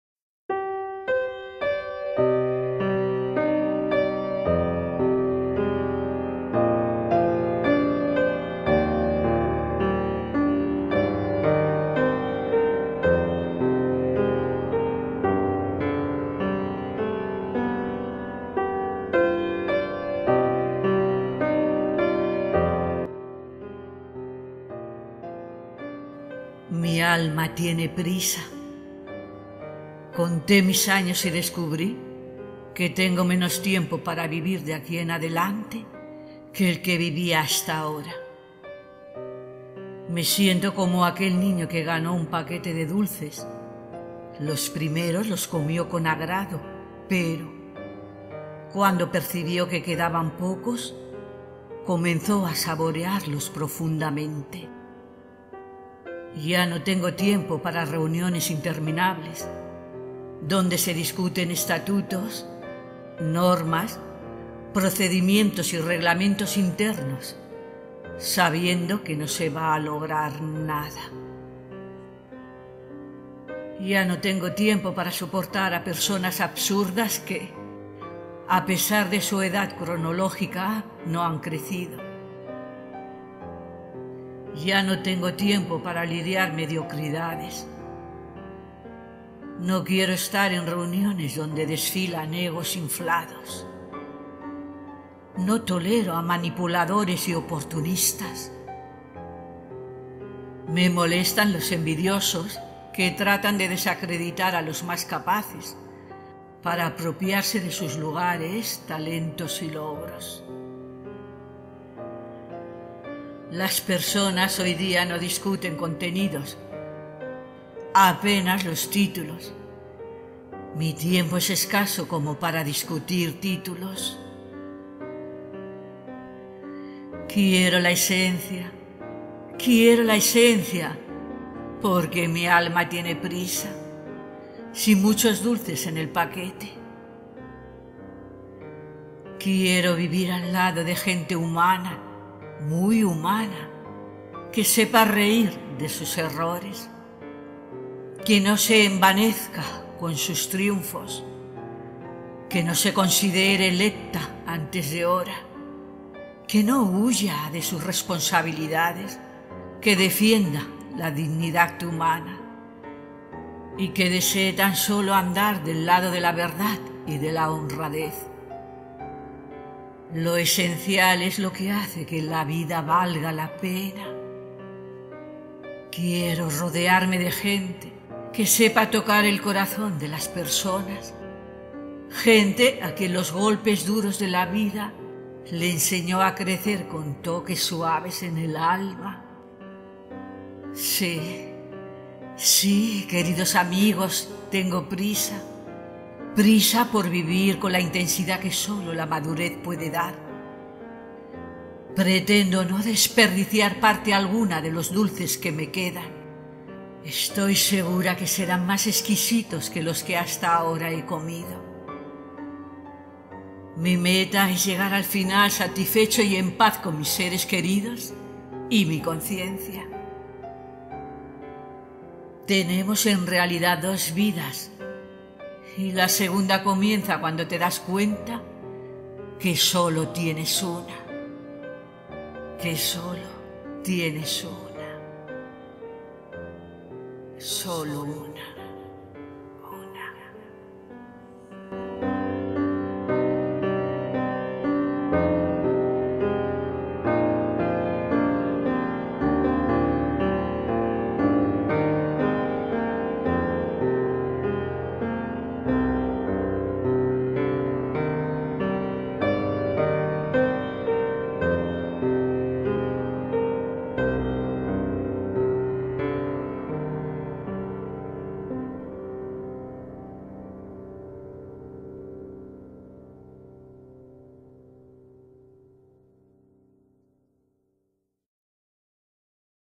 MI ALMA TIENE PRISA (Poema Golosinas) Mário de Andrade Voz Aína
Desde mi corazón os declamo, mis queridos suscriptores, un poema que hace reflexionar, y desde mi ALMA, brotan sentimientos, busco en la poesía emociones y plasmar lo que nos puede tocar el corazón, como el poema de Mário de Andrade "MI ALMA TIENE PRISA". Con el deseo que lo disfruten tanto como yo.

Abrazos envueltos en pétalos de amistad.

Voz, edición: Aína Neruda.

Imágenes sobrepuestas a mi imaginación.

Mário Raul de Morais Andrade (São Paulo, 9 de octubre de 1893 - Ib., 25 de febrero de 1945) fue un poeta, novelista, ensayista, y musicólogo brasileño.

Fue uno de los miembros fundadores del modernismo brasileño.​ En 1922 participó activamente en la Semana de Arte Moderno de São Paulo, que tuvo una gran influencia en la renovación de la literatura y de las artes en Brasil. Su segundo libro de poesía, Paulicéia desvairada, publicado ese mismo año, marca para muchos el inicio de la poesía modernista brasileña.

Durante los años 20 continuó su carrera literaria, al tiempo que ejercía también la crítica musical y de artes plásticas en la prensa escrita. En 1928 publicó su novela más reconocida, Macunaíma, considerada una de las obras capitales de la narrativa brasileña del siglo XX.

From my heart I declare to you, my dear subscribers, a poem that makes you reflect, and from my SOUL, feelings arise, I seek emotions in poetry and express what our hearts can touch, like the poem by Mário de Andrade "MY SOUL HAS HURRY". With the desire that you enjoy it as much as I do.

Hugs wrapped in petals of friendship.

Voice, edition: Aina Neruda.

Images superimposed on my imagination.

Mário Raul de Morais Andrade (São Paulo, October 9, 1893 - Ib., February 25, 1945) was a Brazilian poet, novelist, essayist, and musicologist.

He was one of the founding members of Brazilian modernism, and in 1922 he actively participated in the São Paulo Modern Art Week, which had a great influence on the renewal of literature and the arts in Brazil. His second book of poetry, Paulicéia desvairada, published that same year, marks for many the beginning of Brazilian modernist poetry.

During the 20s he continued his literary career, while also practicing music criticism and plastic arts in the print media. In 1928 he published his most recognized novel, Macunaíma, considered one of the capital works of the Brazilian narrative of the twentieth century.

I counted my years and discovered, that I have less time to live from here on, than the one I lived until now ...

I feel like that child who won a packet of sweets: the first ones ate them with pleasure, but when he perceived that there were few left, he began to savor them deeply.

I no longer have time for endless meetings, where statutes, rules, procedures and internal regulations are discussed, knowing that nothing will be achieved.

I no longer have time to support absurd people who, despite their chronological age, have not grown up.

I do not have time to deal with mediocrity.

I do not want to be in meetings where inflated egos parade.

I do not tolerate manipulators and opportunists.

I am annoyed by the envious, who try to discredit the most capable, to appropriate their places, talents and achievements.

People do not discuss content, just the titles.
My time is short to discuss titles.

I want the essence, my soul is in a hurry ...

Without many sweets in the package ...

I want to live next to human, very human people.
That knows to laugh, of its errors.
Do not be conceited, with their triumphs.
Dont consider elect before time.
Do not let him get away from his responsibilities.
That defends, the human dignity.
And want to just walk on the side of truth and honesty.

The essential is what makes life worthwhile.

I want to surround myself with people who know how to touch the hearts of people ...
People to whom the hard blows of life, taught him to grow with soft touches in the soul.

Yes ... I'm in a hurry ... to live with the intensity that only maturity can give.

I intend not to waste any part of the sweets that I have left ...
I'm sure they'll be more exquisite than what I've eaten so far.

My goal is to reach the end satisfied and at peace with my loved ones and with my conscience.
We have two lives and the second one begins when you realize that you only have one.